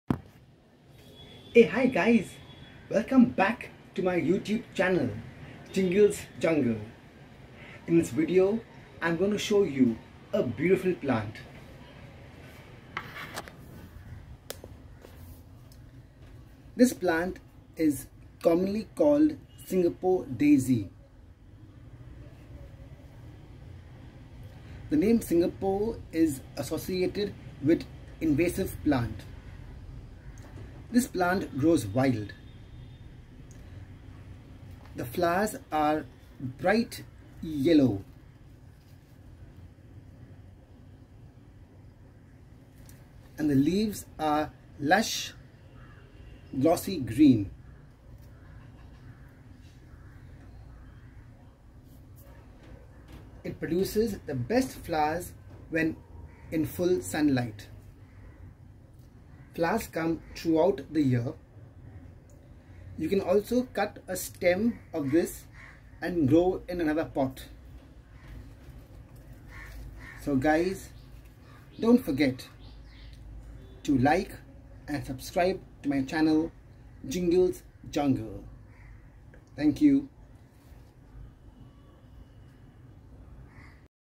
Singapore Daisy
The name Singapore is associated with invasive plant. The flowers are bright yellow and leaves are lush glossy green.